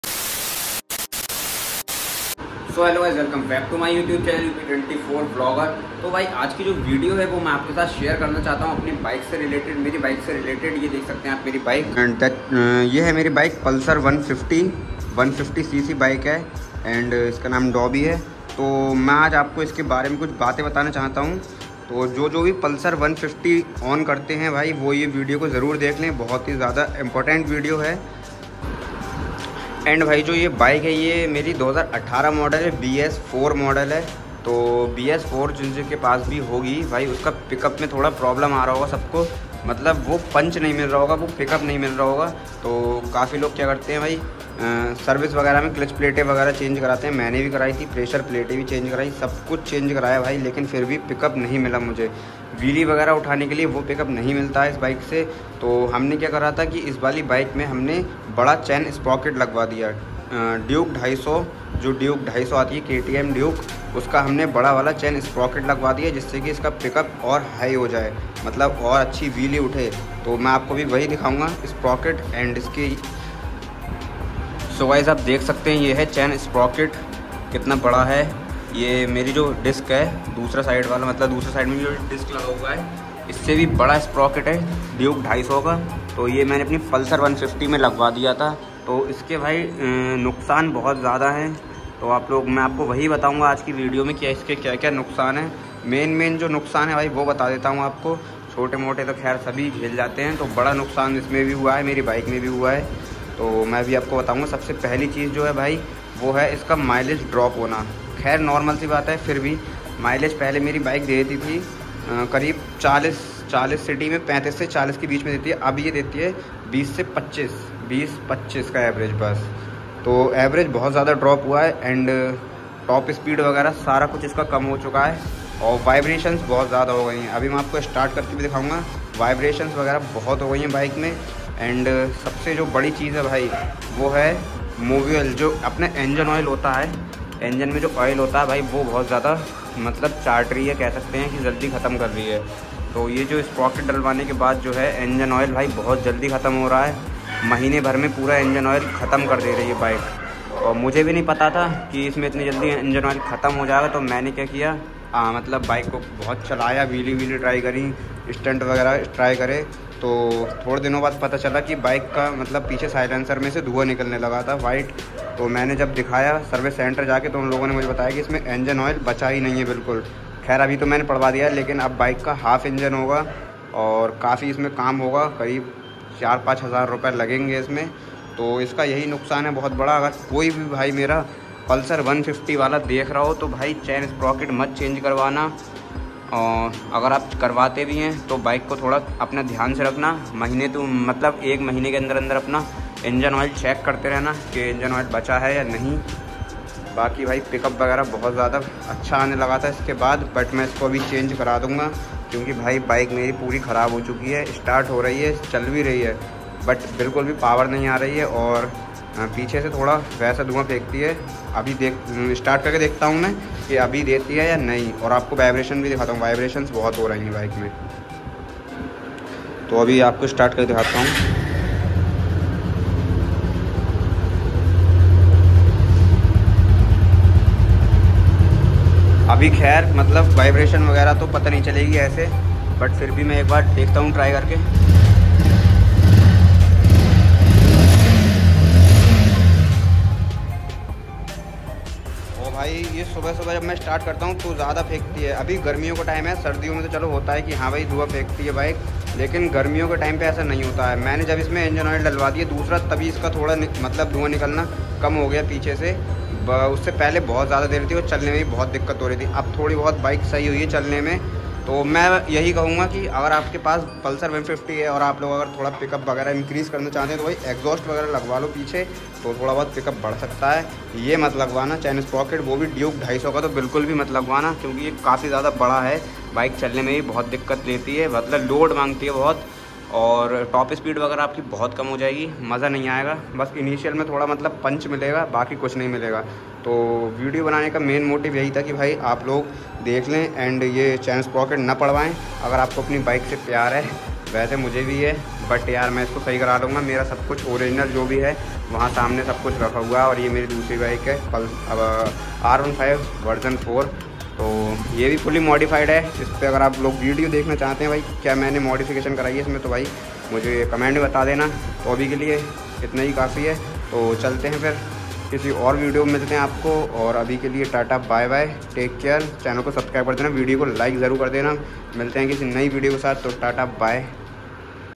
Yeh galti mat krna kabhi | bajaj pulsar 150 bs4 pickup problem 🥲
Yeh glti app na kre vrna bike ke engine main dikkt hi skti hai jaise mere me hogai hai 🥲

Video banane ka main motive tha ki app sabko apna experience share kar sakuu ✌️

Share karo apne pulsar vale dosto ko 💫